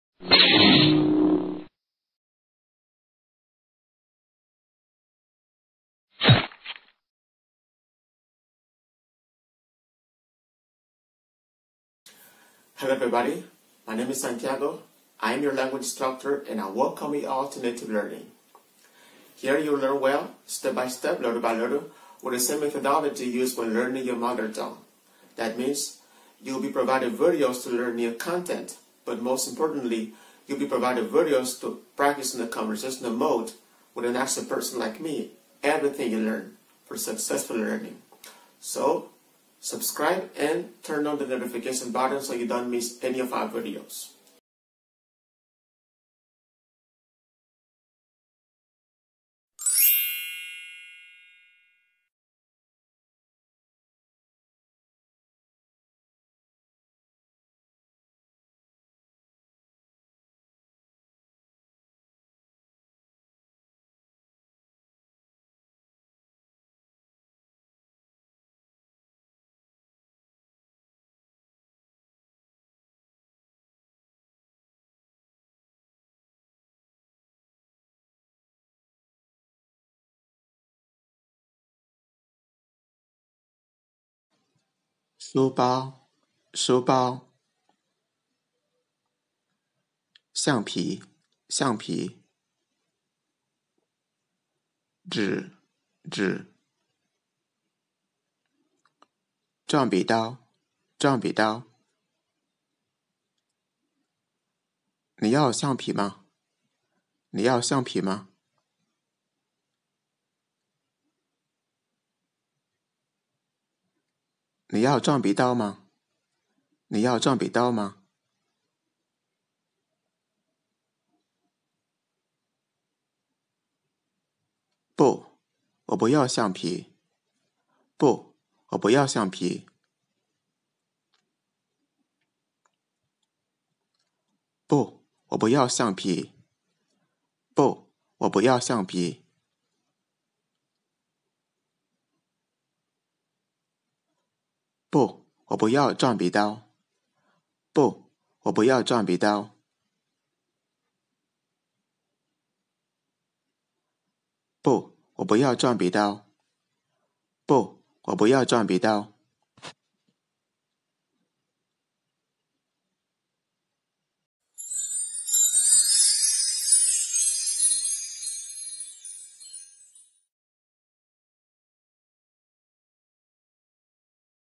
Learn Chinese talk about classroom tools 3/3.
In this video you will learn to talk about some classroom tools. NATIVE LEARNING.